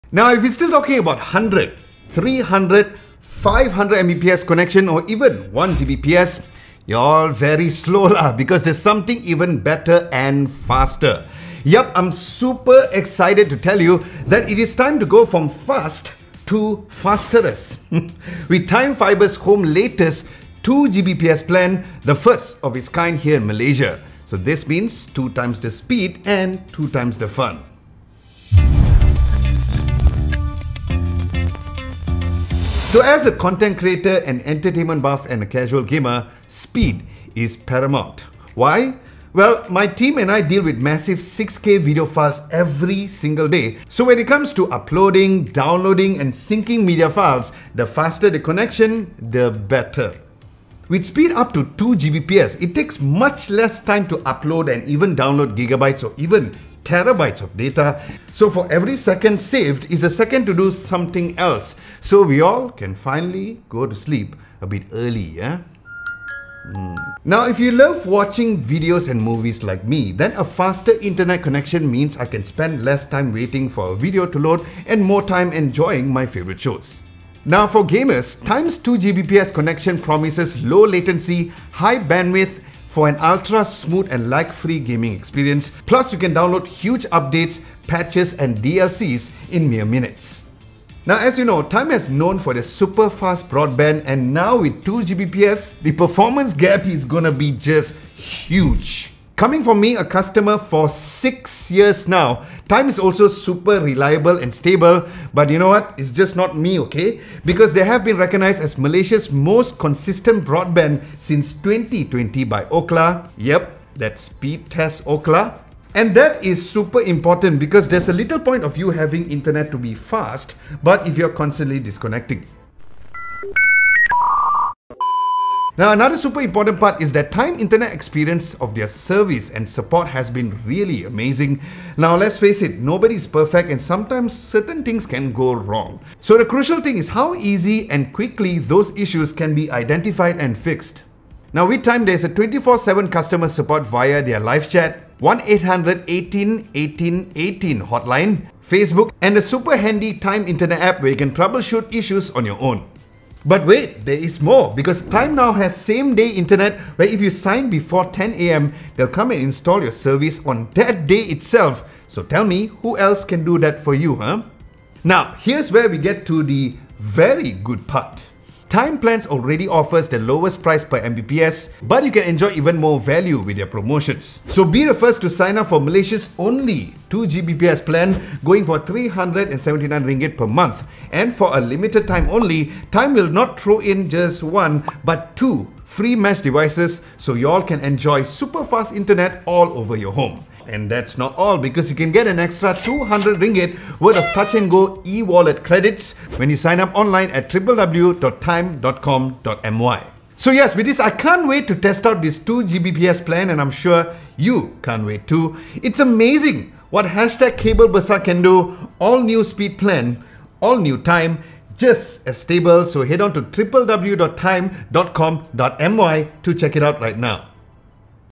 The fasterest Internet plan in Malaysia! | Kabel Besar 2Gbps
We love Adam Lobo’s enthusiasm! Watch him go from fast to fasterest as he tries out Malaysia’s only 2Gbps plan, with Time Fibre Home.

And you could also be the fasterest by checking out if 2Gbps is in your area